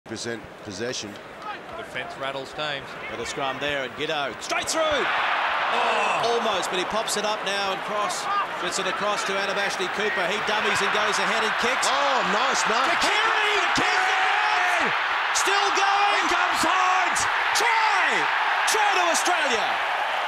A 2008 Wallabies classic 😮‍💨
A classic Wallabies try back in 2008 against the All Blacks!

Watch the full game on our channel!